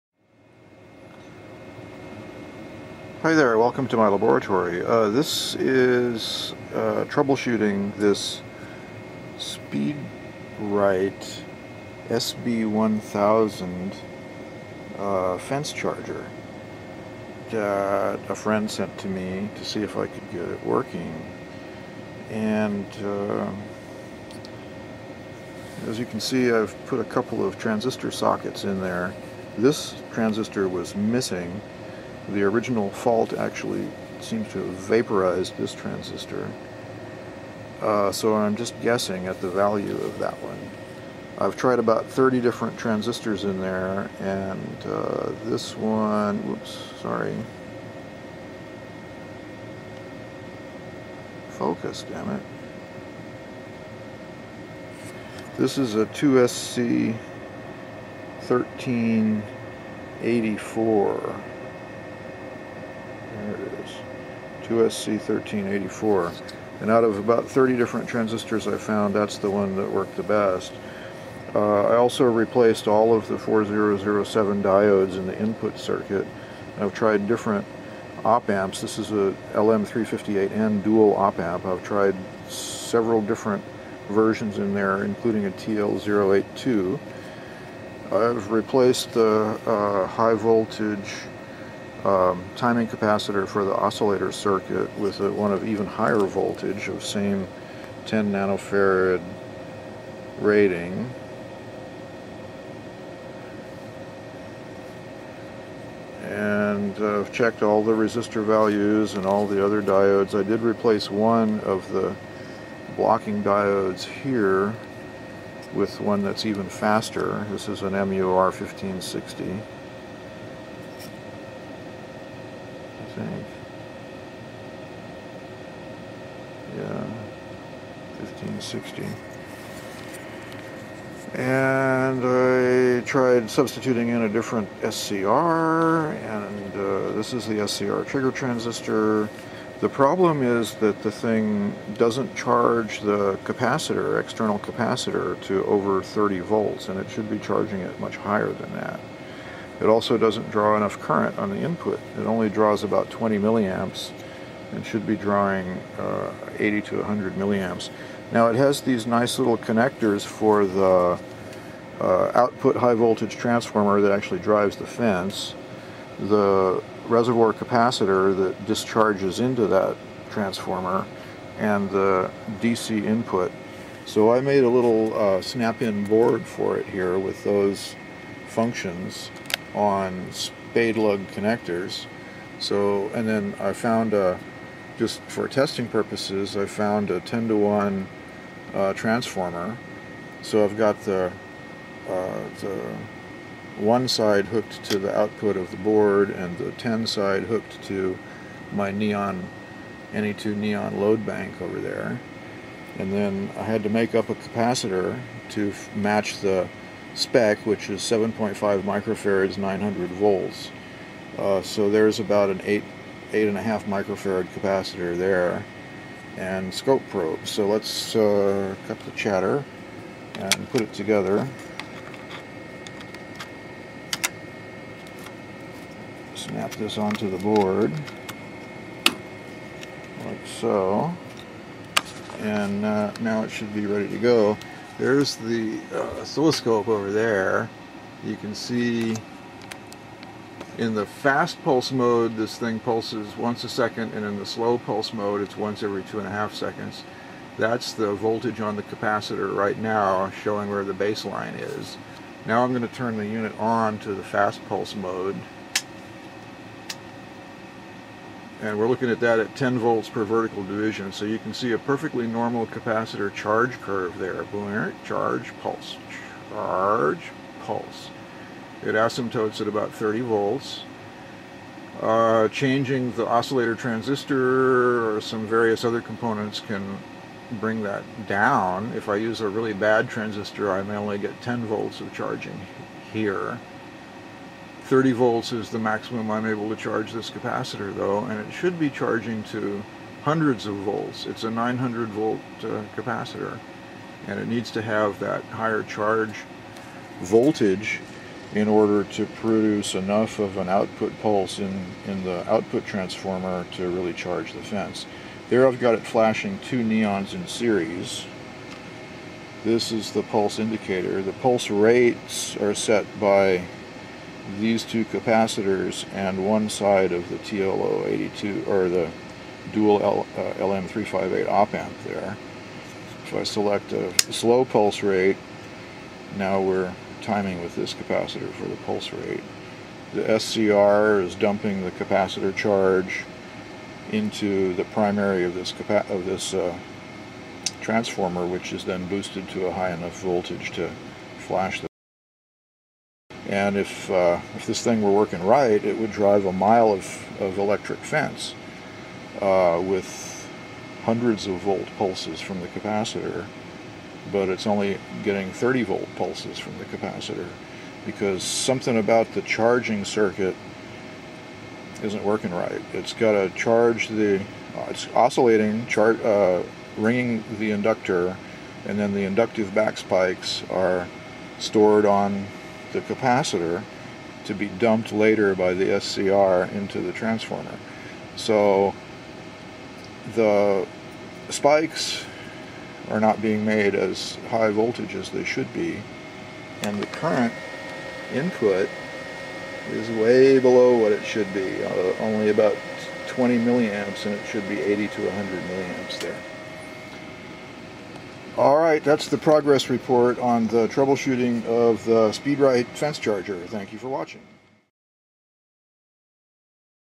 Troubleshooting and Repair: SpeedRite SB1000 Fence Charger
A friend sent me this malfunctioning fence charger module to see if I could get it working.

And I have done, sort of. I'm working without a schematic or parts list, without the HV output transformer and the pulse capacitor, but I've made substitutes for those parts which are probably adequate for testing. The unit is still not charging the capacitor properly although all other functions seem to be working as designed.

If anyone out there can provide me with a schematic with parts list, or even just the right part number for the Q2 transistor, that would be very much appreciated.
The transistor that I thought was a 2sc556b which triggers the SCR is actually a PNP, BC556B, a real cheapie. I've tried other pnps here but I can't get them to pulse, although they do allow charging.

This is a SpeedRite SB1000 fence charger, originally made in 1994 or perhaps 1996.